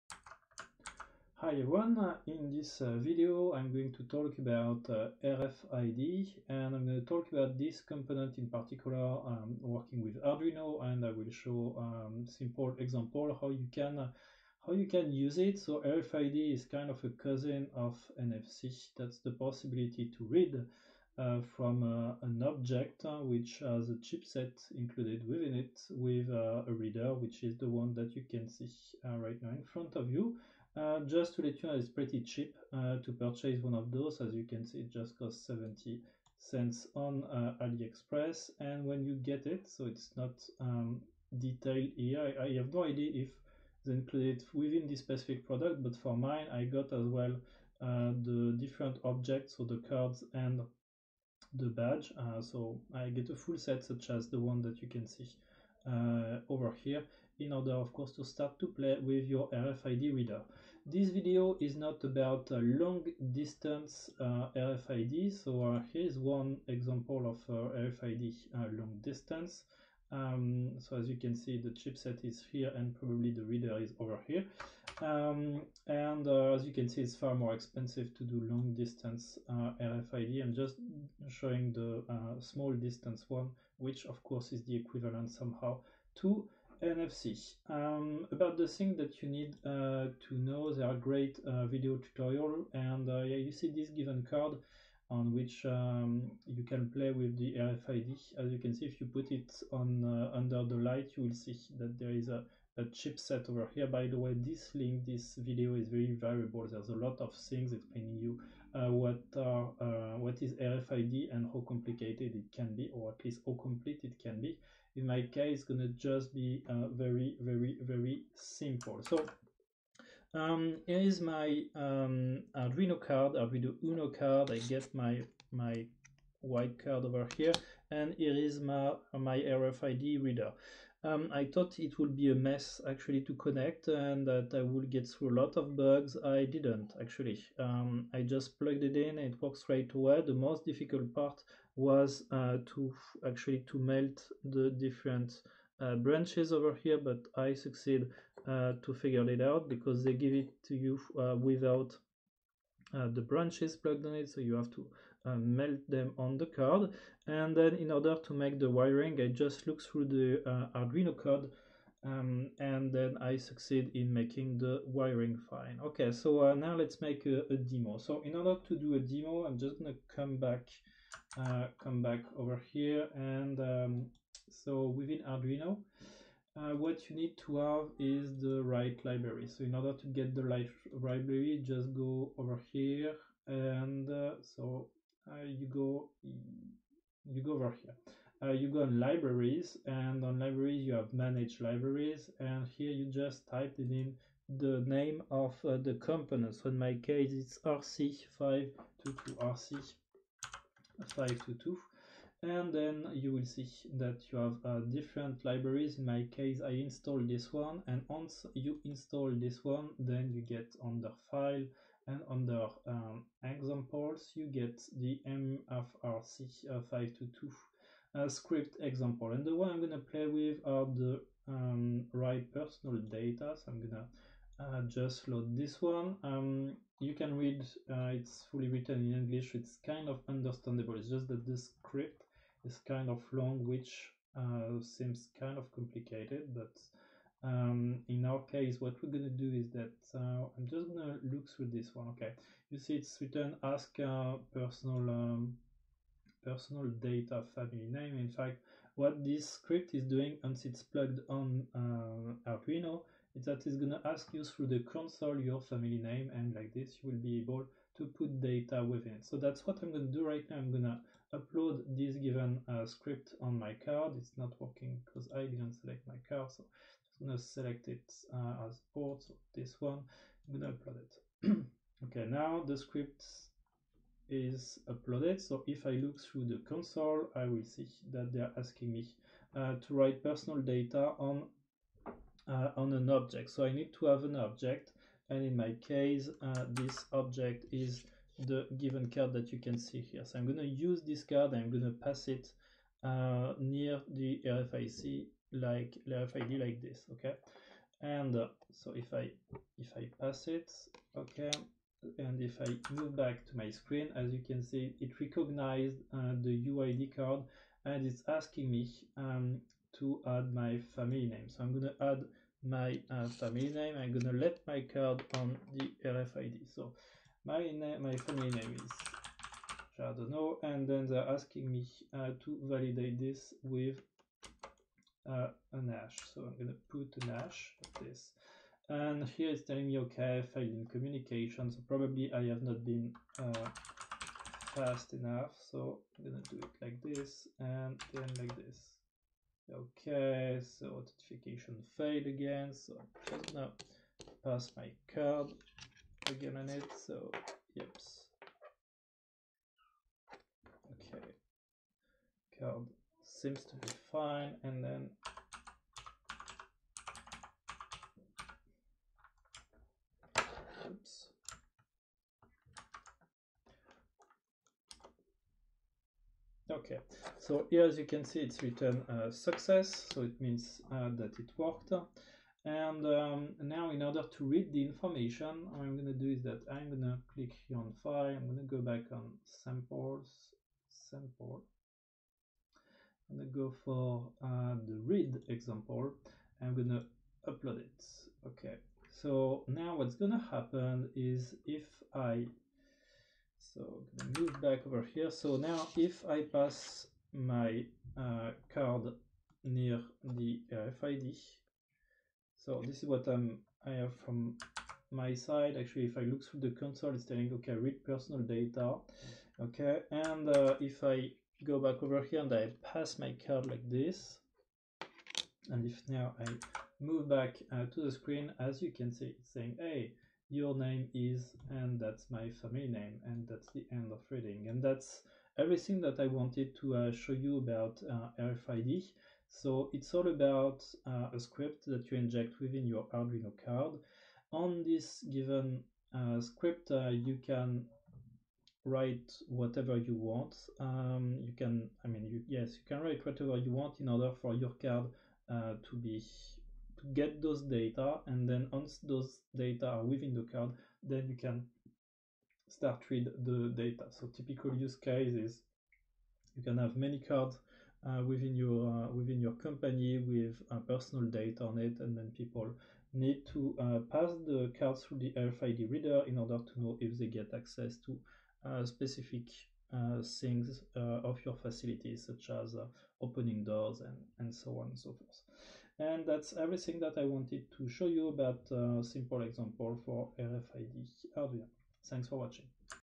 Simple example of Arduino with RFID
A RFID example by using Arduino. This video is part of the IOT course within the FLOSS Marketing School.